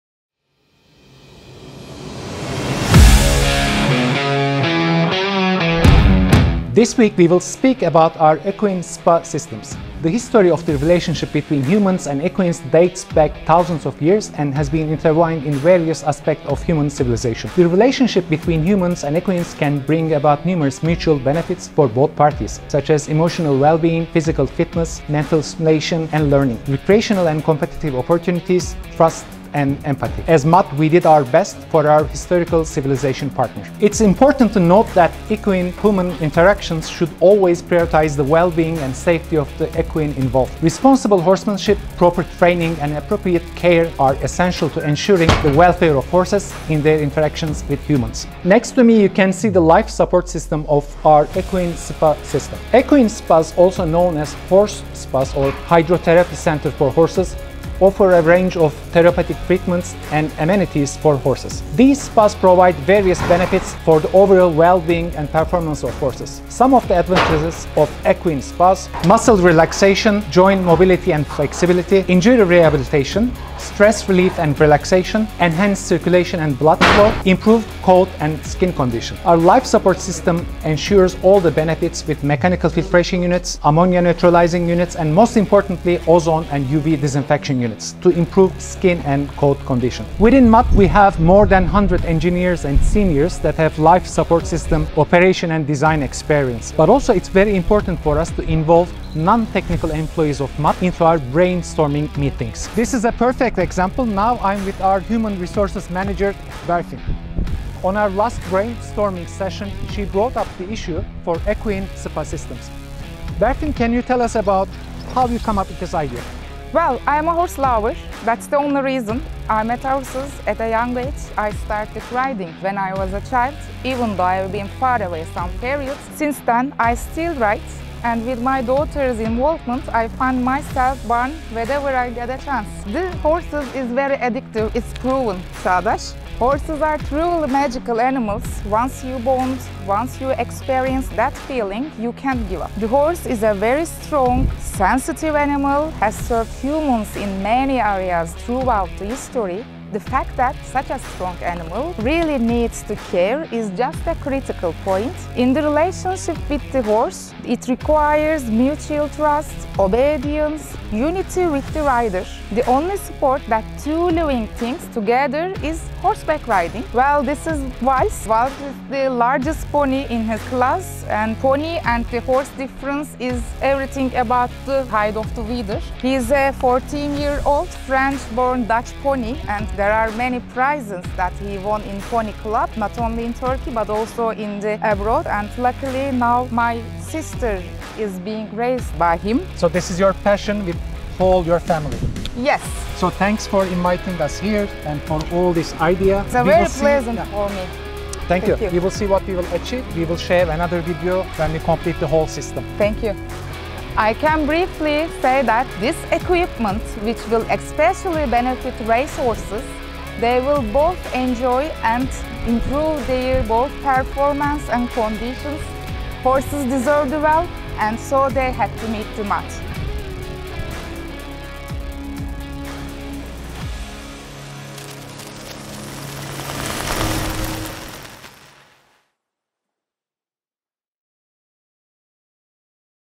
Equine Spa Filtration System (Horse Water Treatment Skid)
Equine spas, also known as horse spas or hydrotherapy centers for horses, offer a range of therapeutic treatments and amenities for horses. These spas provide various benefits for the overall well-being and performance of horses.

Our life support system ensures all the benefits with mechanical filtration units, ammonia neutralizing units and most importantly ozone and UV disinfection units to improve skin and hair condition. Besides our expertise on aquatic life its also our passion to deliver best for all animals.